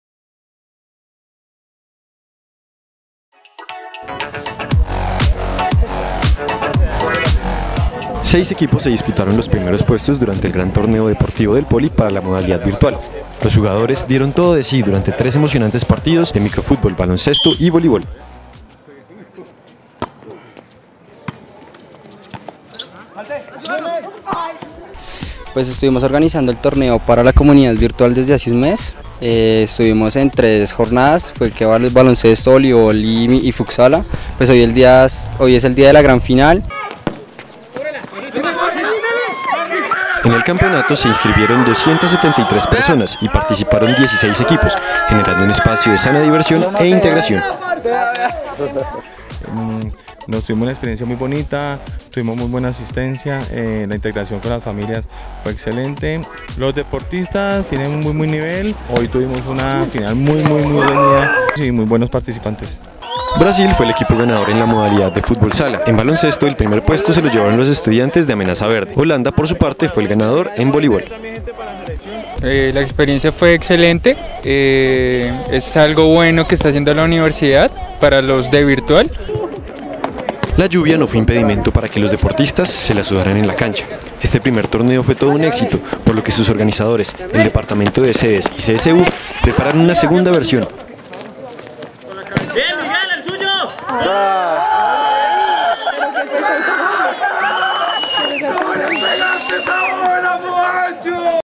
Las finales del 1er Torneo para Estudiantes de la Modalidad Virtual
Fueron tres apasionantes encuentros en los cuales se disputaron los títulos de campeón y subcampeón de microfutbol, baloncesto y voleibol. Estos son algunos de los mejores momentos.